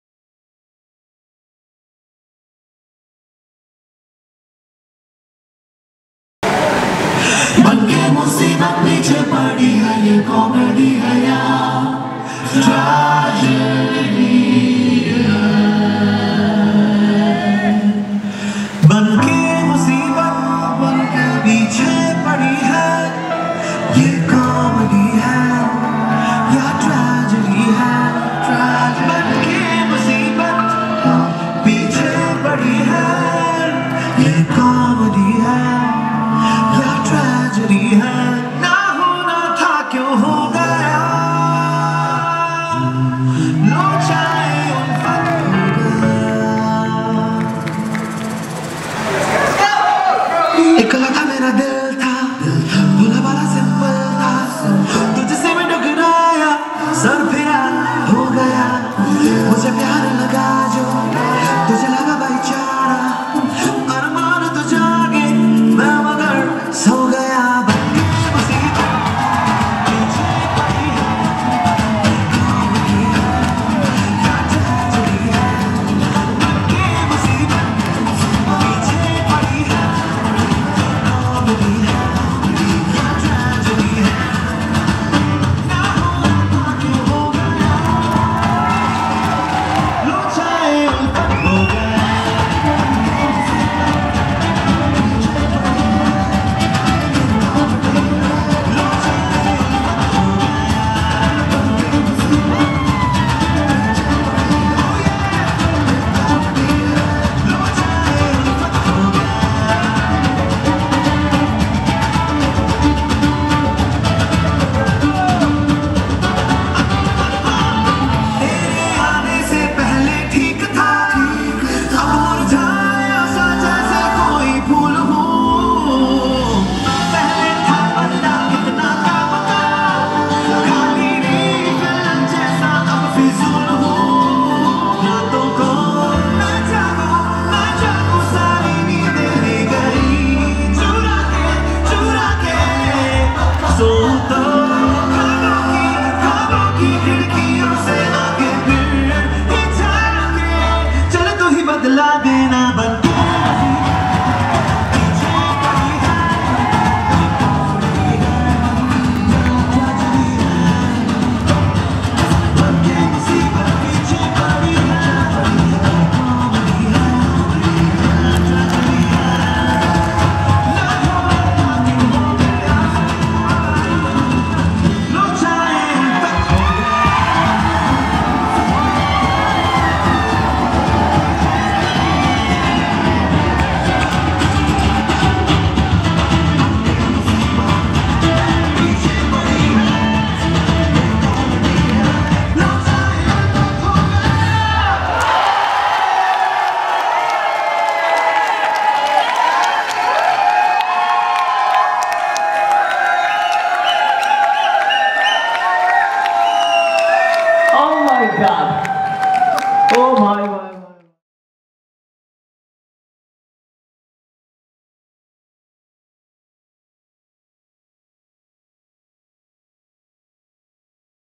Supahit FAM.O.U.S Crew | Judge Showcase | Indian Krump Festival 9 2019 | TheVerb Official
INDIAN KRUMP FESTIVAL also known as IKF is one of India's biggest krump event. It is also known as the home for Indian krumpers. A platform for all the Indian krumpers by organizing workshops and battles as international krumpers as faculty. This initiative was started by ASURA and FAMOUS CREW. After 8 successful editions, they have successfully completed the 9th one.

More Love from TheVerb Studio